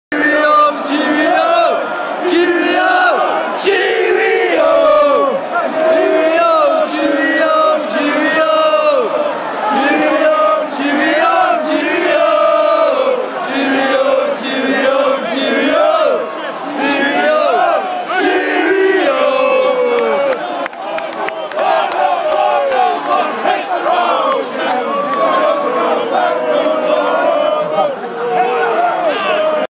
Scarlets fans to Ospreys fans (cheerio cheerio!)
Thousands of Scarlets fans stood up and chanted 'cheerio cheerio cheerio' to the Ospreys fans leaving the stadium early due to losing.

The Scarlets won the match 22 - 10